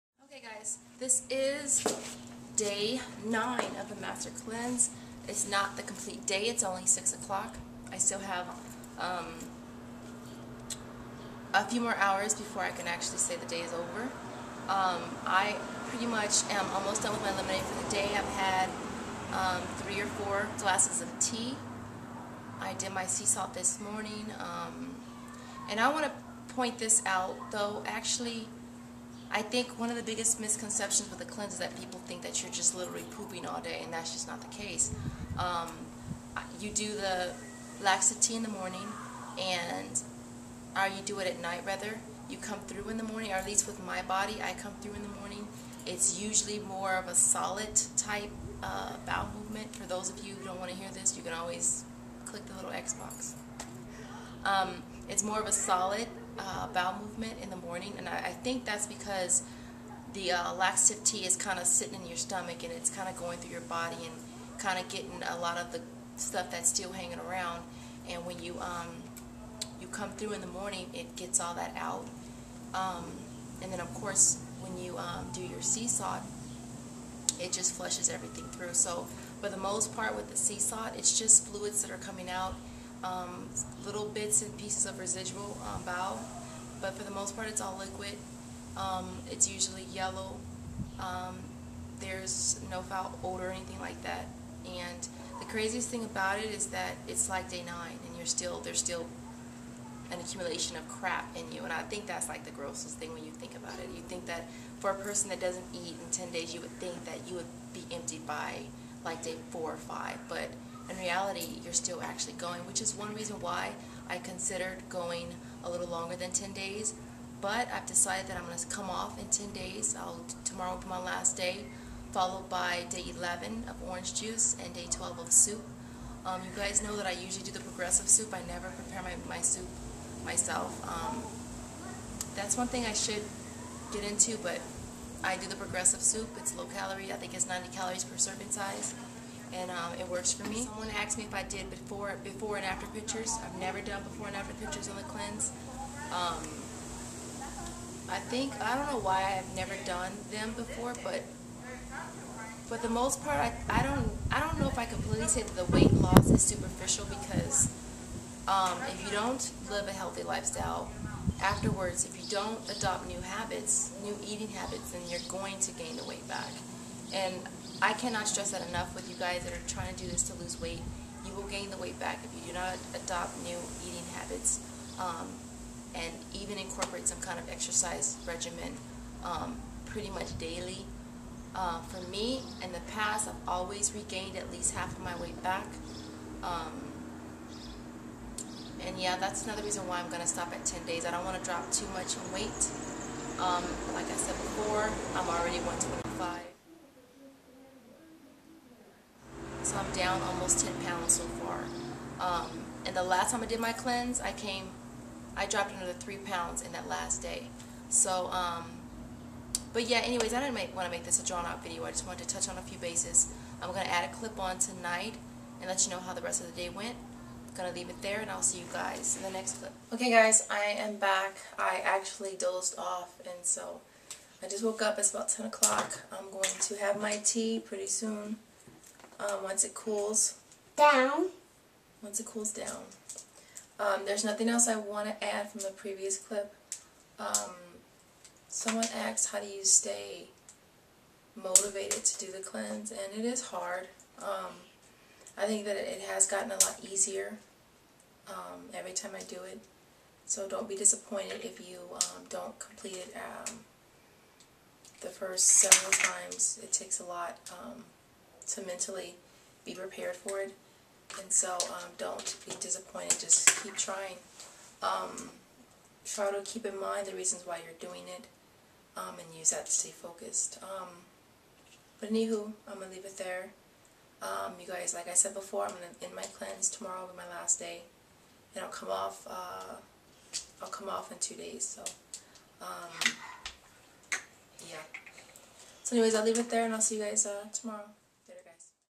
Master Cleanse Day 9 ( ok so yea I'm still pooping, lmfao)
today was day 9, one more day left. My current weight is 124.8 so I'm down almost 10 lbs. Gonna jus do 10 days because I don't wanna get to low in weight.